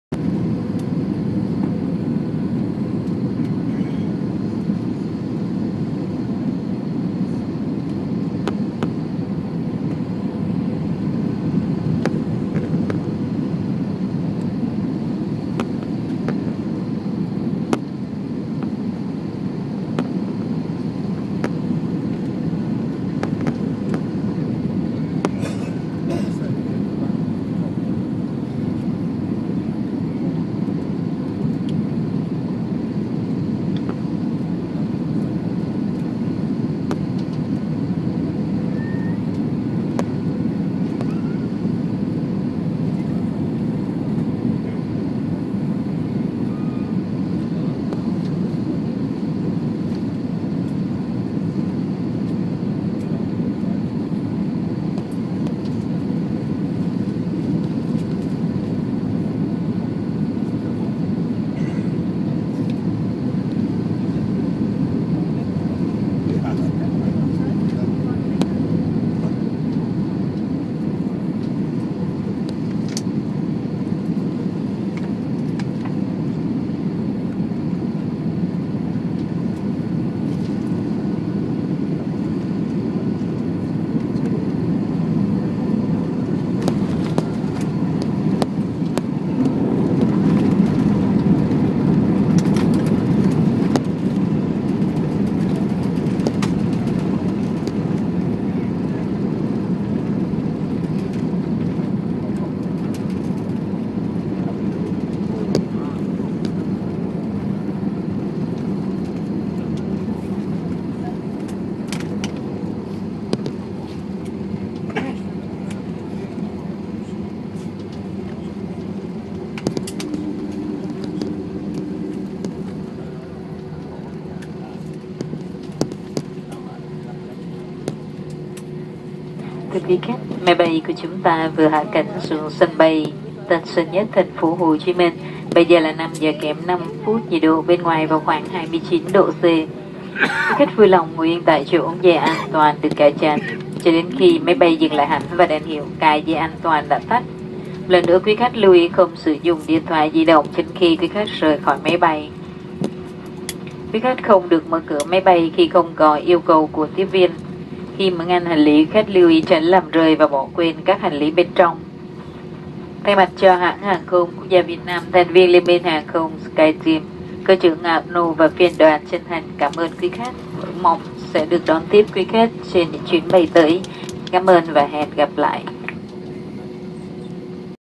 Vietnam Airlines Airbus A321 Landing in Ho Chi Minh City from Qui Nhon [VN1397]
Vietnam Airlines Airbus A321 landing in Ho Chi Minh City Tan Son Nhat International Airport after a short domestic flight from Qui Nhon, Vietnam as VN1397.